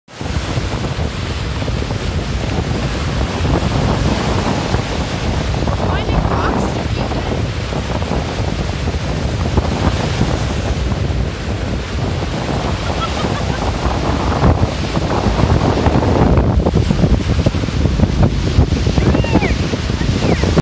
German Shepherd Playing on the Beach
This is Logan, back at the beach. This time though, he is older and wiser. Logan understands his foe well and has brought new strategies at attacking the water!

Logan is a purebred Czech German Shepherd. He is now a year old and his red colours are starting to show through.